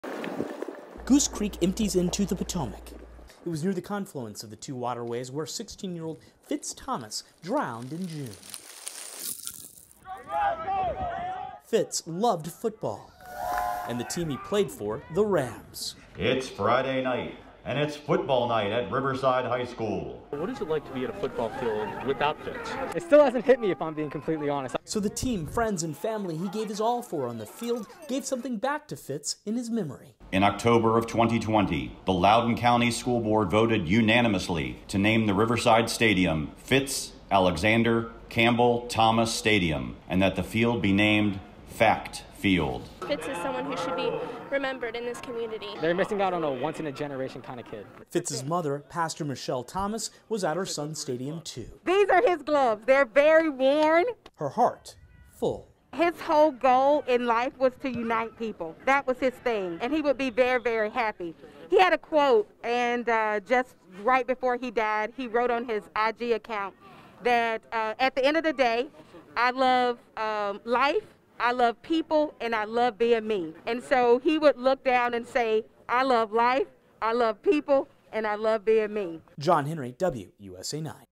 Loudoun high school football team honors former player who died last year
Riverside High School played its first football game at the newly named Fitz Alexander Campbell Thomas Stadium Friday since the passing of 16-year-old Fitz Thomas.